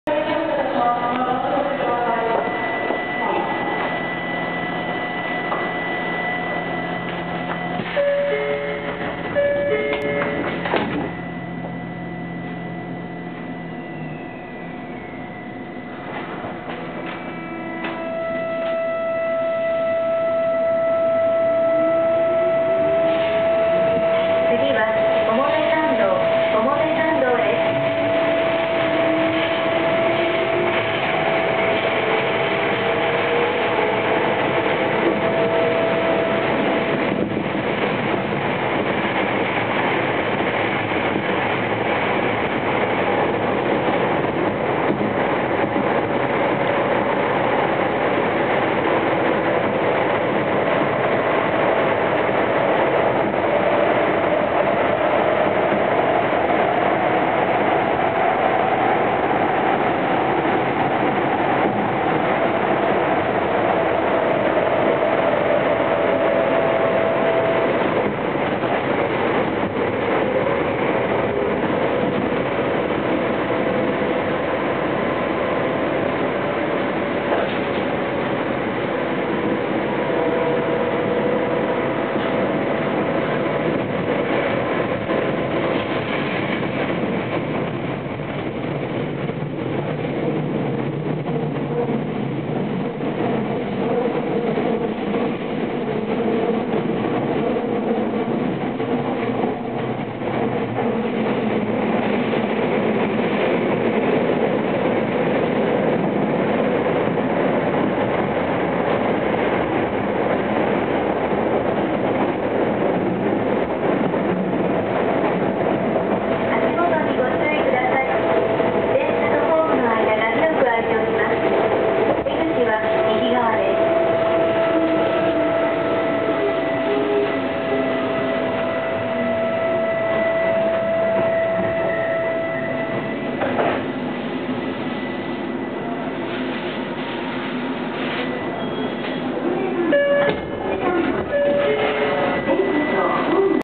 東京メトロ6000系 6335 乃木坂→表参道
東京メトロ6000系最終増備車の走行音です。ドアチャイムとLED案内表示器搭載、車外スピーカーが搭載されたといった内装中心に重点を置きました。三菱GTOチョッパ制御ですが、綺麗に3段階鳴っていないようです。